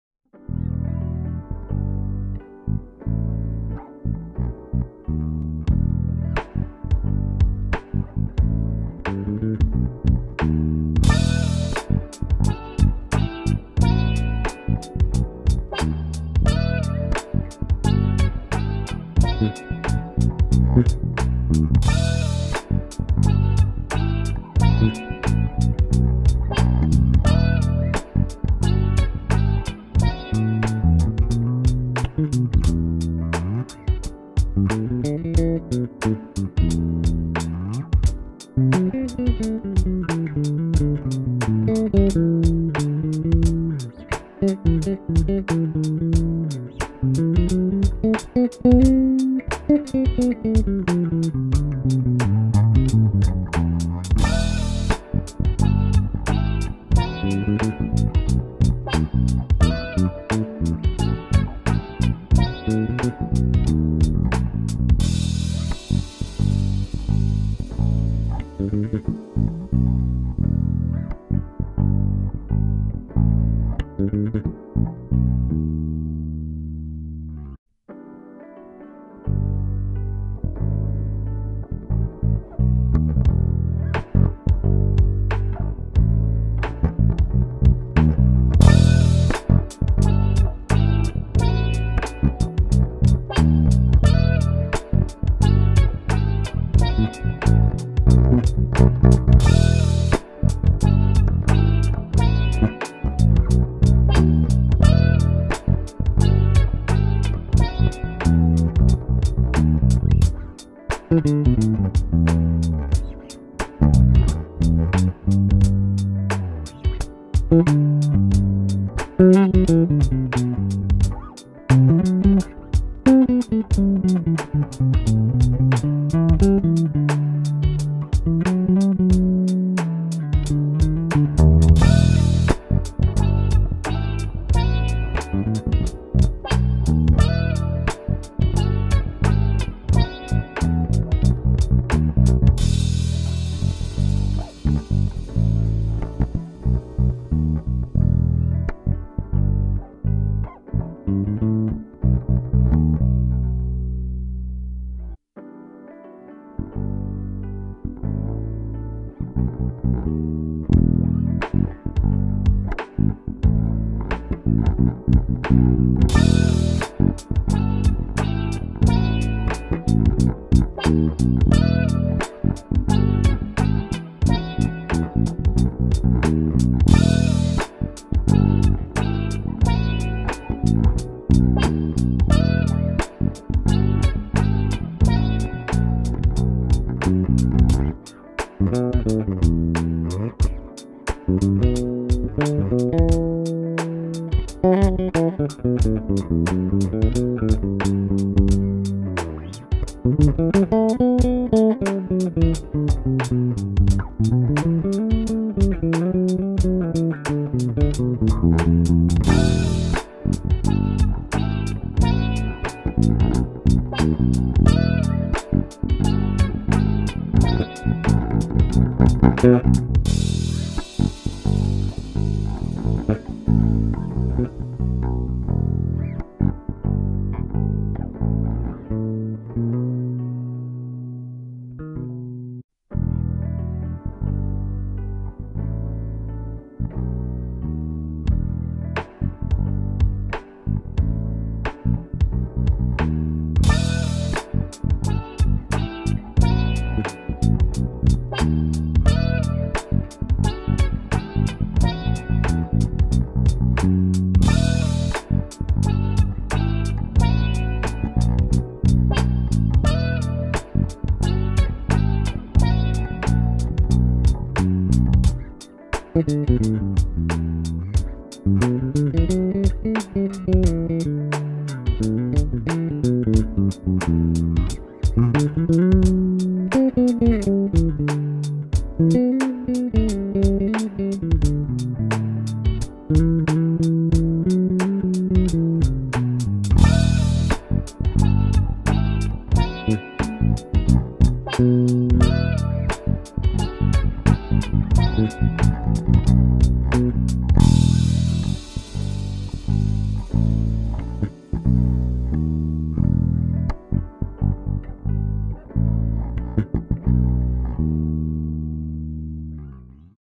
elrick goldseries soundcheck 弾き比べてみた
使用機材
Erick goldseries 4
  Bartolini soapbar
  bartolini preamp
 hartke bass attack
 steinberg neve ur-rt2

トラックメーカー
Ryoma

He is very young track maker.
but his song is good groove, and good passion!
if you want to contact, search his instagram&send message.
He wants to make many track!